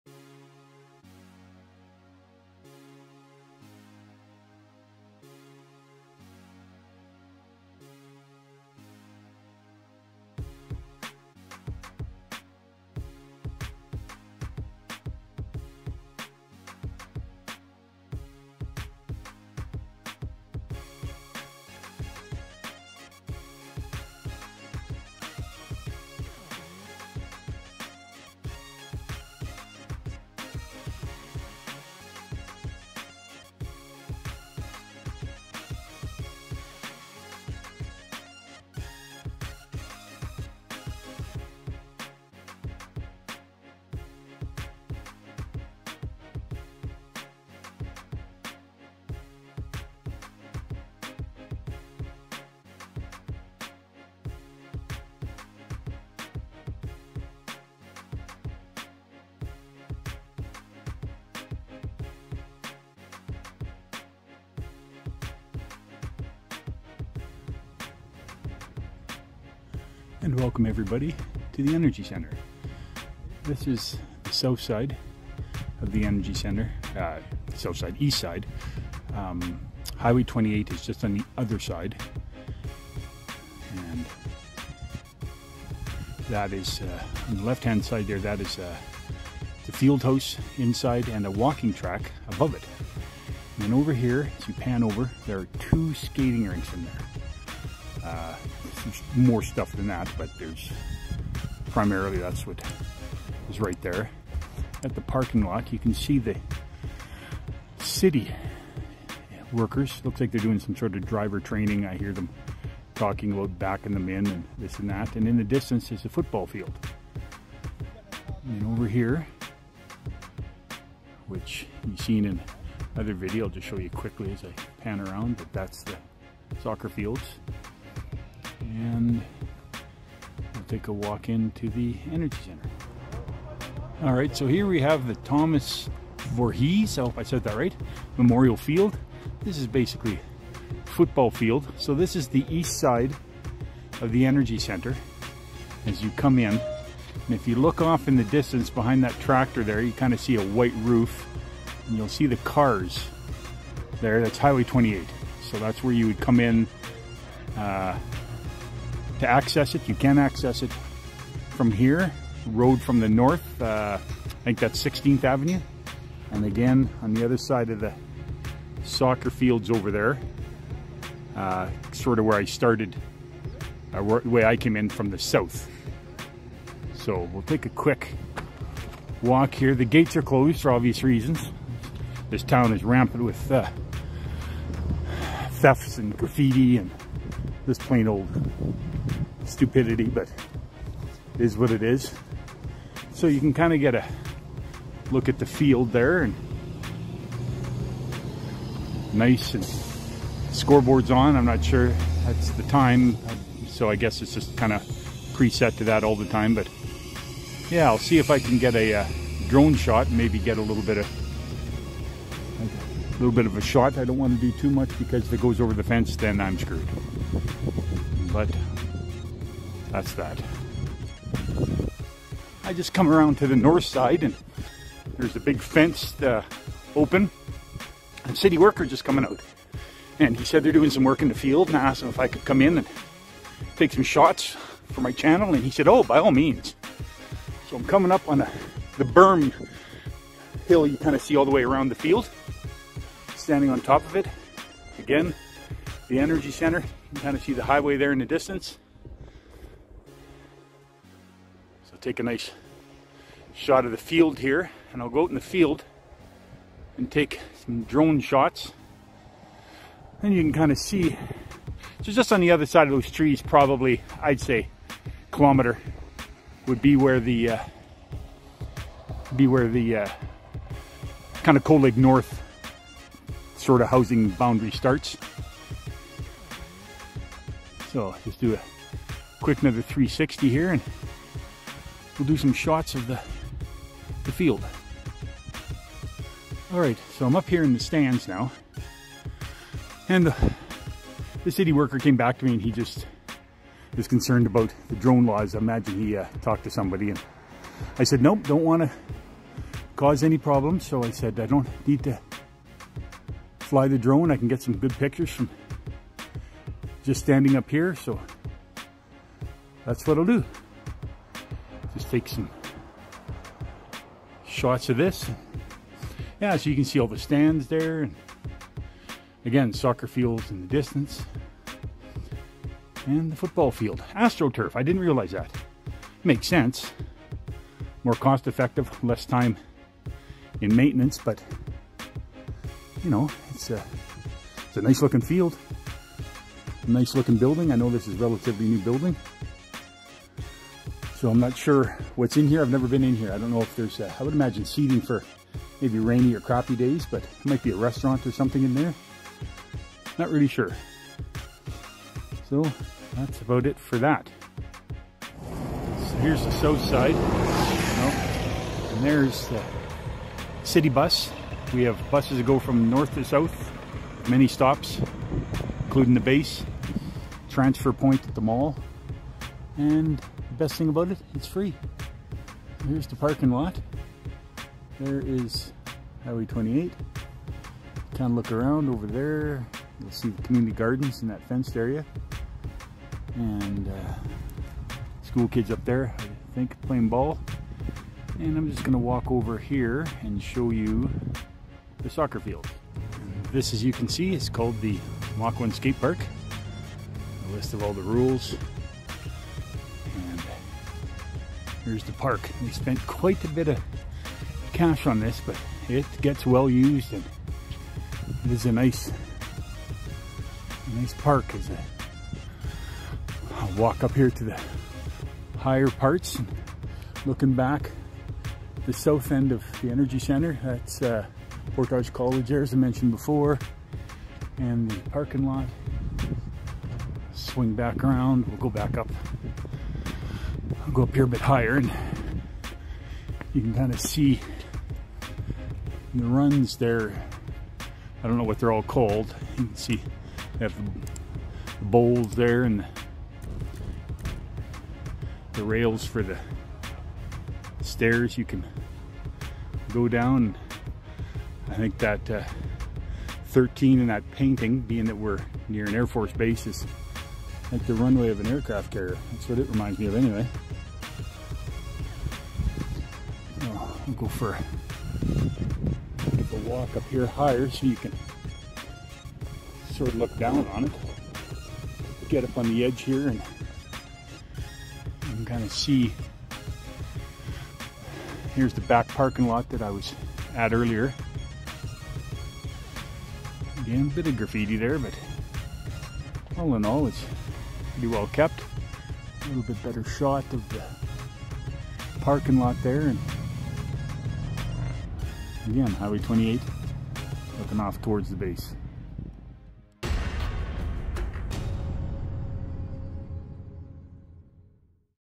Cold Lake: Part 7 - Energy Centre
This is just the outside grounds as I am waiting permission to film on the inside so another video is coming.